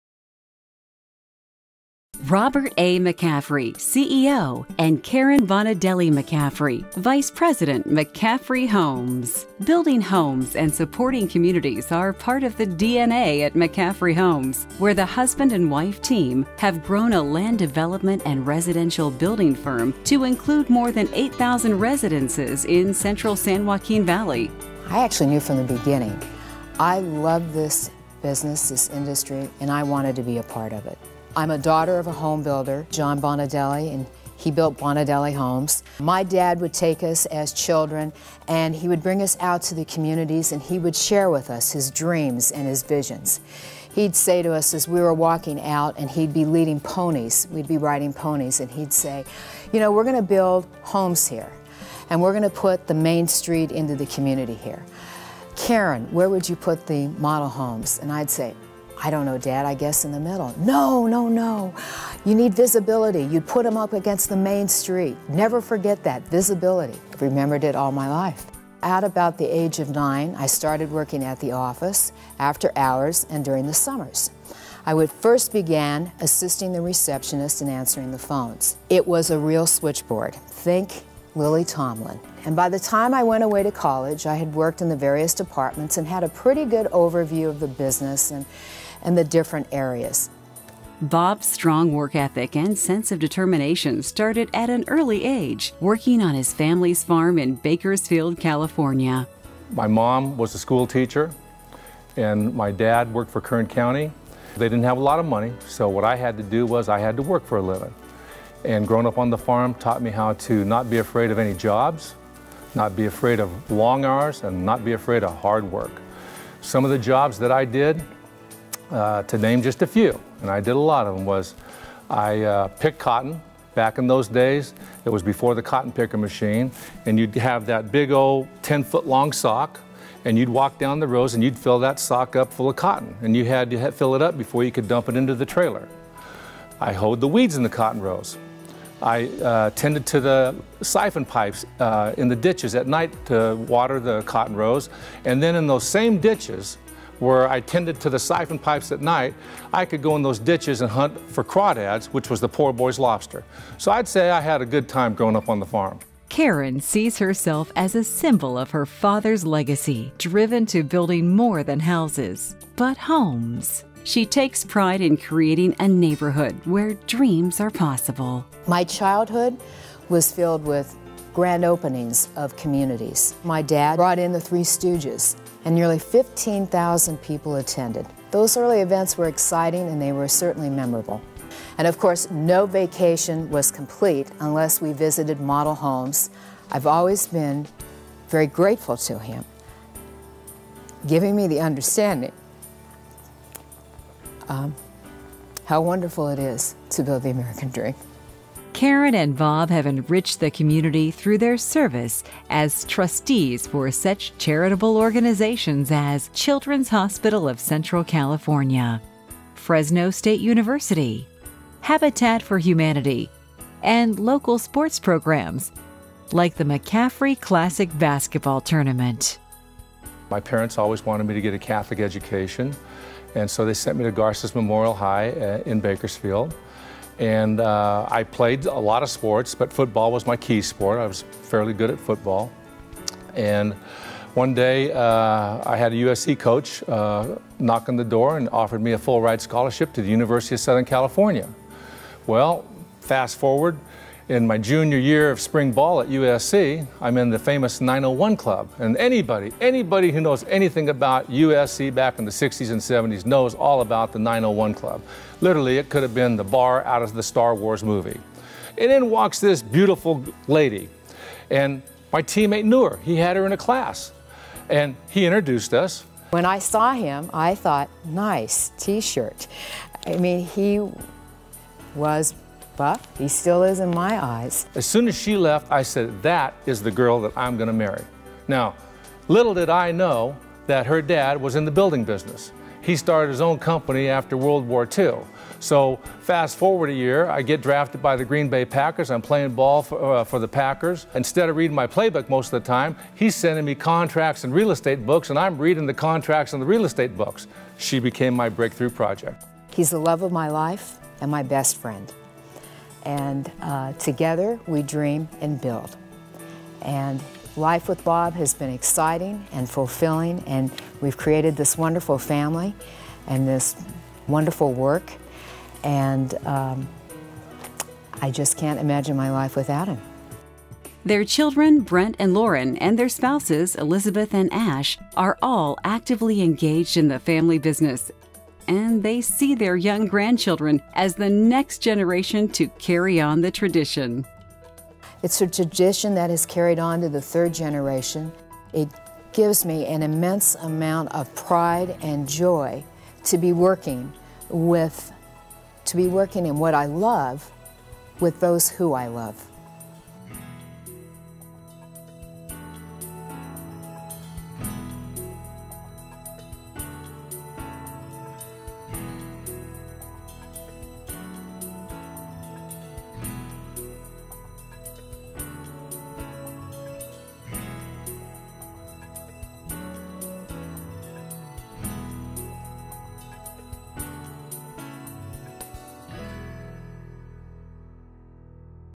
California Homebuilding Foundation's Hall of Fame - The McCaffrey's Induction video
California Homebuilding Foundation's Hall of Fame - 30 years

Narrator:
Robert A. McCaffrey CEO and Karen Bonadelle McCaffrey VP, McCaffrey Homes.

Building homes and supporting communities are part of the DNA at McCaffrey Homes. Where the husband-and-wife team have grown a land development and residential building firm to include more than 8000 residences in central San Joaquin Valley.

Karen's Story:
Karen:
I actually knew from the beginning. I love this business, this industry and I wanted to be part of it. I'm a daughter of homebuilder, John Bonadelle, and he built Bonadelle Homes. My dad would take us as children and he would bring us out to the communities, and he would share with us his dreams and his visions.

He'd say to us as we were walking out and he'd be leading ponies - we'd be riding ponies.
And he'd say, "You know we're going to build homes here, and we're going to put the main street into the community here. Karen, where would you put the model homes?"
And I'd say, "I don't know Dad I guess in the middle?"
"No no no! You need visibility.  You put them up against the main street. Never forget that, visibility."

I've remembered it all my life.

At about the age of nine I started working at the office, after hours and during the summers. I first began assisting the receptionist and answering the phones. It was a real switchboard -  think Lily Tomlin - and by the time I went away to college I had worked in the various departments and had a pretty good overview of the business and the different areas.

Bob's Story:
Narrator:
Bob's strong work ethic and sense of determination started at an early age working on his family's farm in Bakersfield, California.

Bob:
"My mom was school teacher and my dad worked for Kern county. They didn't have a lot of money so what I had to do was I had to work for a living. And growing up on the farm taught me how to not be afraid of any jobs, not be afraid of long hours, and not be afraid of hardwork.

Some of the jobs that I did, to name just a few and I did a lot of them, was I picked cotton - back in those days, it was before the cotton picking machine and you'd have that big ol' 10 foot long sock and you'd walk down the rows and you'd fill that sock up full of cotton, and you had to fill it up before you could dump it into the trailer.  I hoed  the weeds in the cotton rows.

I tended to the siphon pipes in the ditches at night to water the cotton rows and then in those same ditches where I tended to the siphon pipes at night, I could go on those ditches a hunt for crawdads which was the poor boys lobster.

So I'd say I had a good time going up on the farm."
Narrator:
Karen sees herself as a symbol of her father's legacy driven to building more than houses but homes.  She takes pride in creating a neighborhood where dreams are possible.

Karen:
My childhood was filled with grand openings of communities. My dad brought in the Three Stooges and nearly 15,000 people attended. Those early events were exciting and they were certainly memorable.

And of course no vacation was complete unless we visited model homes. I've always been very grateful to him for giving me the understanding... How wonderful it is to build the American Dream.

Narration:
Karen and Bob have enriched the community through their service as trustees for such charitable organizations as Children's Hospital of Central California, Fresno State University, Habitat for Humanity, and local sports programs like the McCaffrey classic basketball tournament.

(cont)